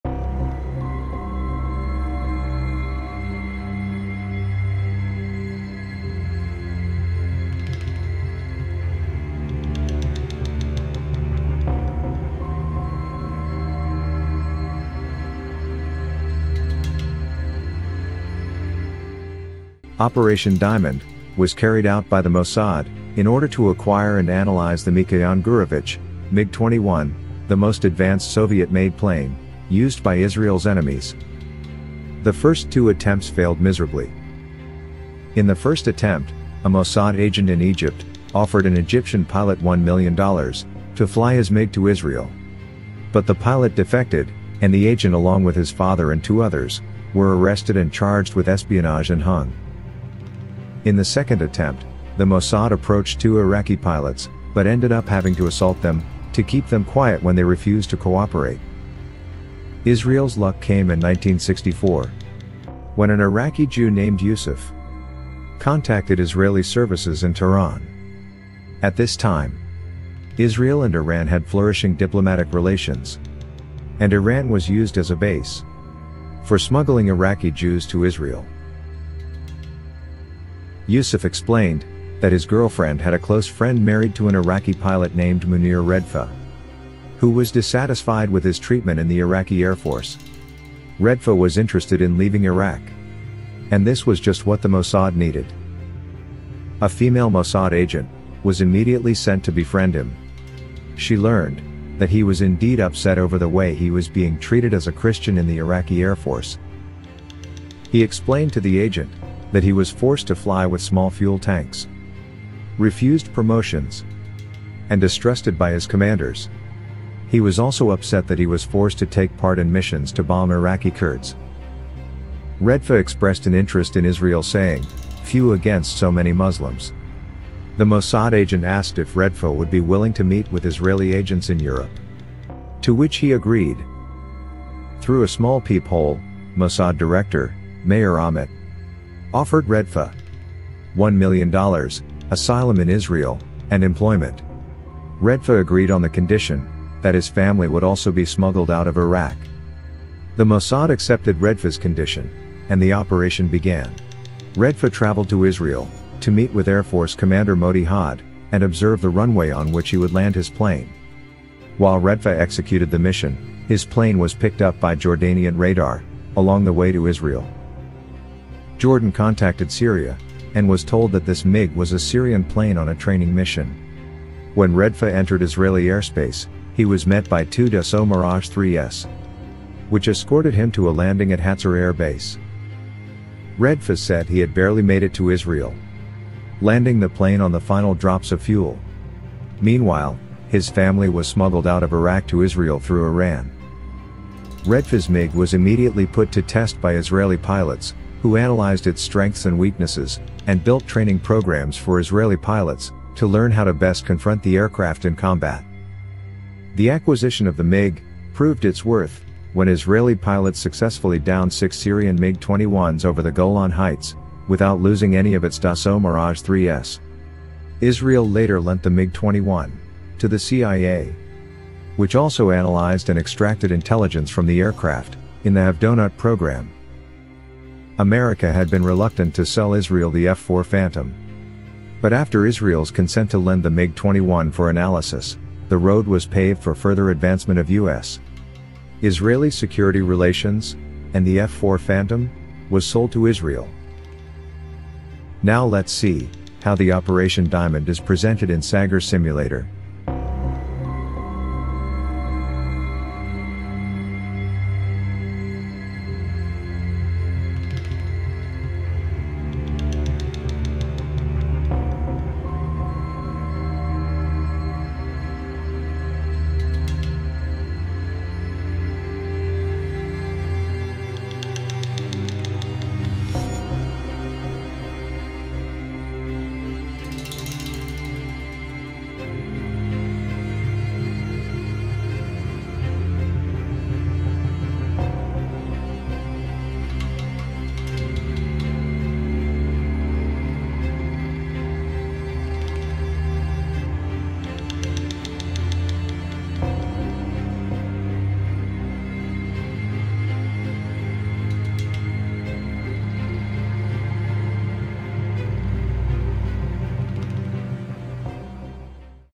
Operation Diamond' 1964 / Israeli Air Force / Sagger Military Actions
Sagger Military Actions is a series of videos about battles, military operations and actions.
In this video we would like to show you some background information about Operation Diamond, carried out by the Mossad, that had positive influence on the Yom Kippur War.

To find out more about Yom Kippur War check out our free demo version of Yom Kippur War Simulator: